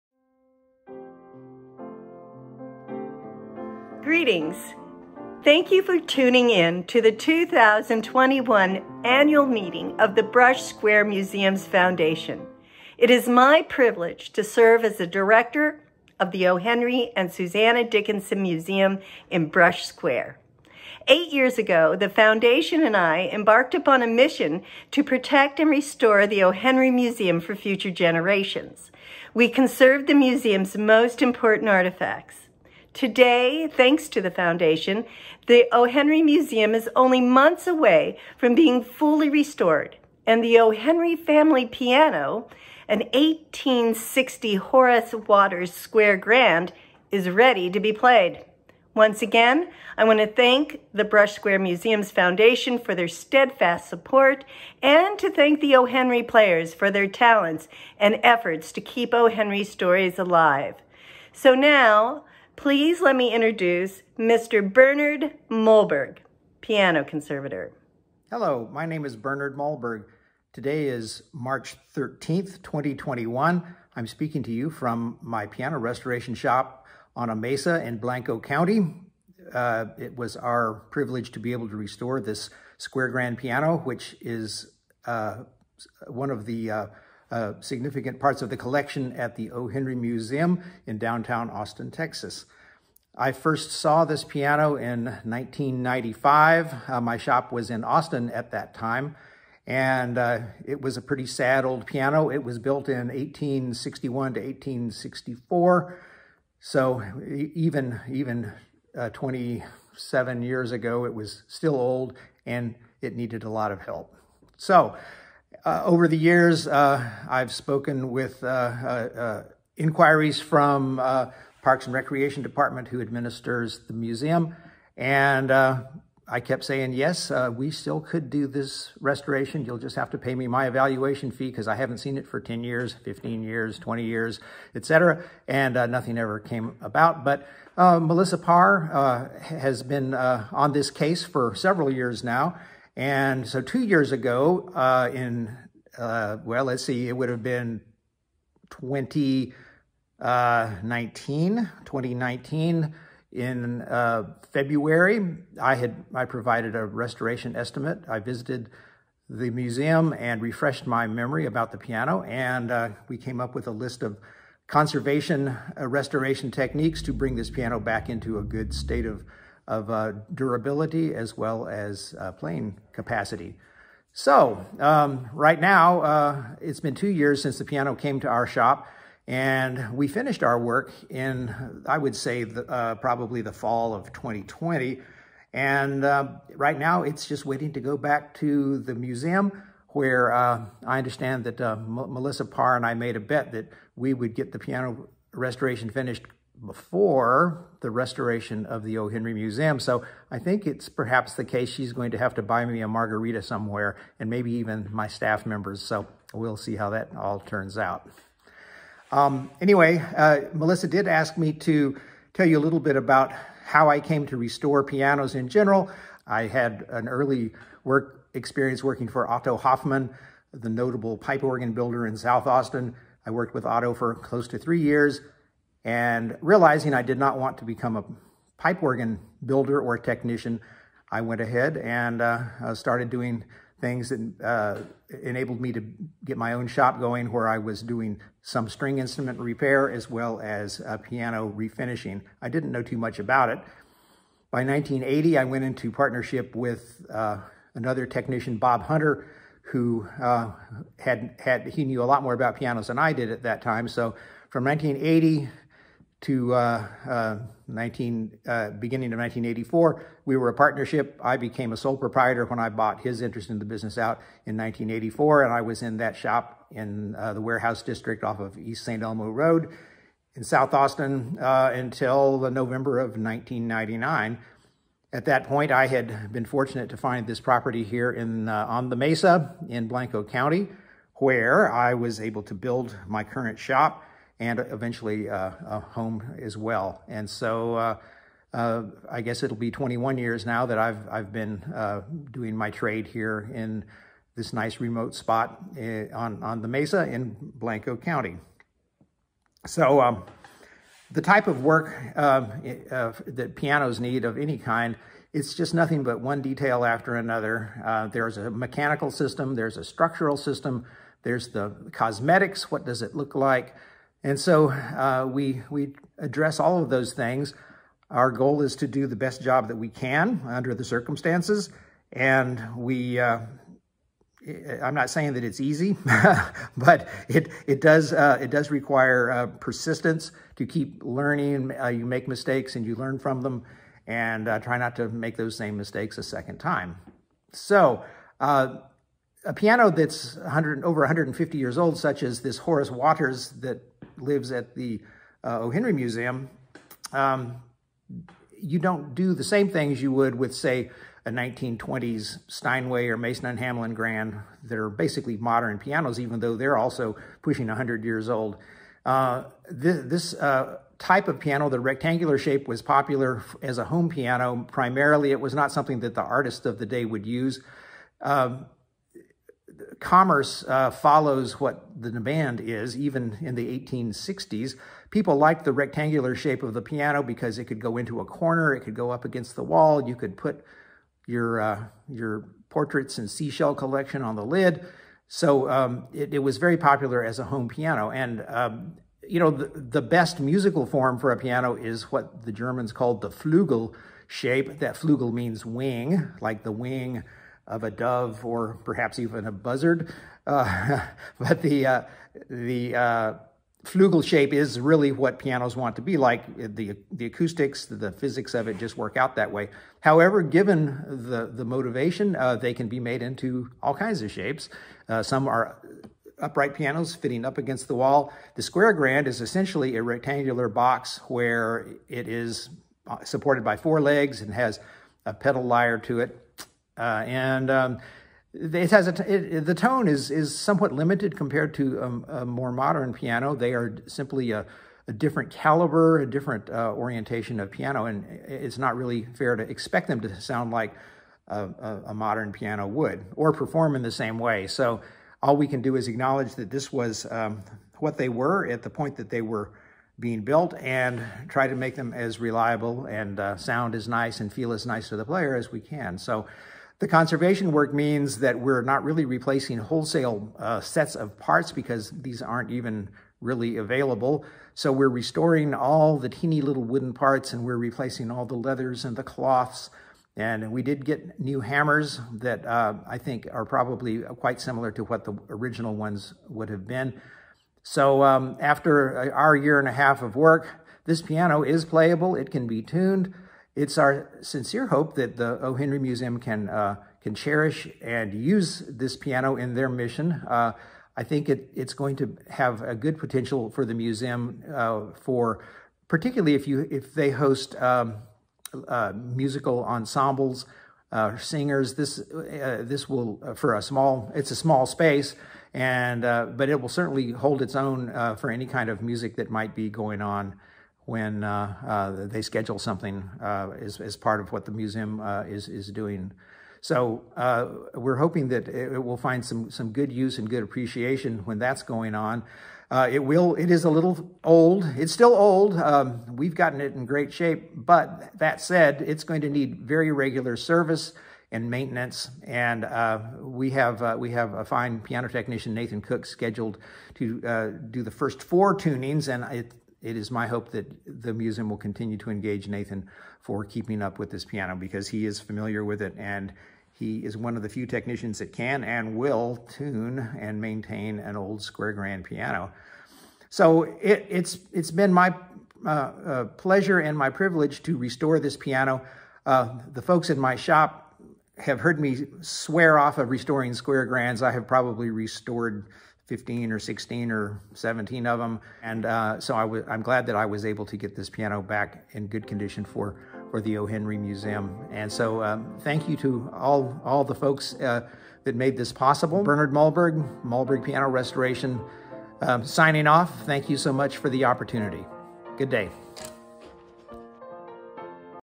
Restoring The O. Henry Museum Square Grand Piano
The story of how the 155-year-old Horace Waters square grand piano from the O. Henry Museum, Austin, Texas was restored in 2020 to playable condition, as told by Bernard Mollberg owner of Mollberg Piano Restorations. Introduction by Melissa Parr, Director of Brush Square Museums.